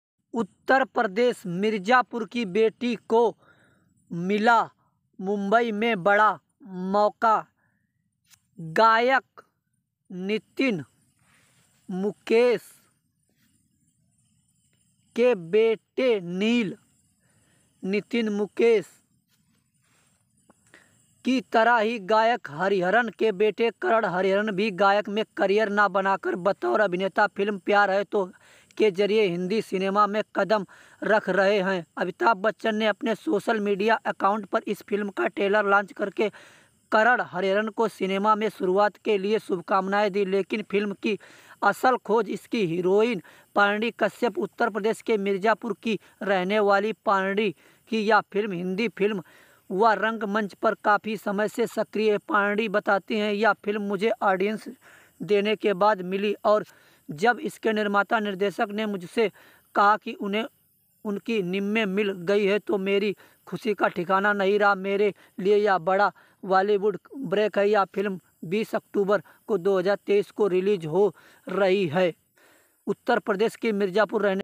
मिर्जापुर की बेटी को मिला मुंबई में बड़ा मौका पाणि कश्यप फिल्म का ट्रेलर लॉन्चअमिताभ बच्चन
traileमिर्जापुर की बेटी को मिला मुंबई में बड़ा मौकाr